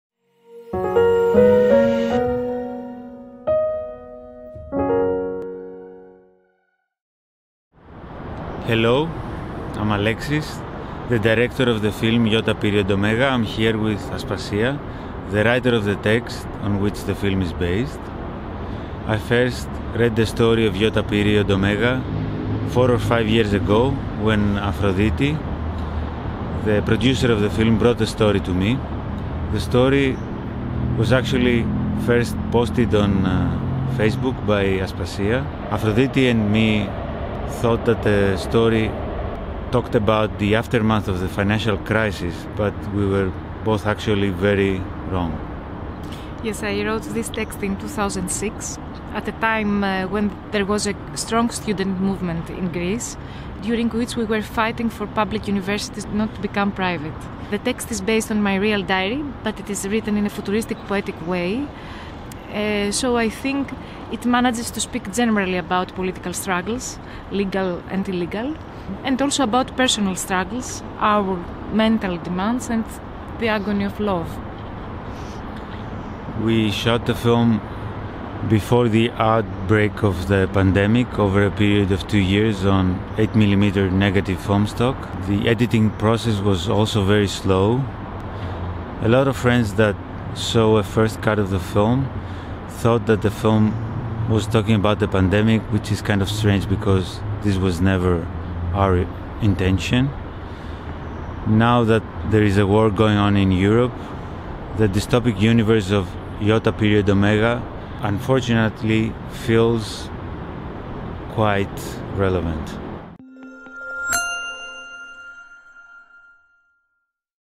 [2022 BISFF] 이오타 피리어드 오메가 Iota Period Omega l 알렉시스 알렉시우Alexis Alexiou
국제경쟁 9
International Competition 9

제목 Title l 이오타 피리어드 오메가 Iota Period Omega
감독 Director l 알렉시스 알렉시우 Alexis Alexiou

제39회 부산국제단편영화제에서 경쟁 감독님들이 전하는 인사를 공개합니다.
Warm greetings from directors of competitons at 2022 BISFF.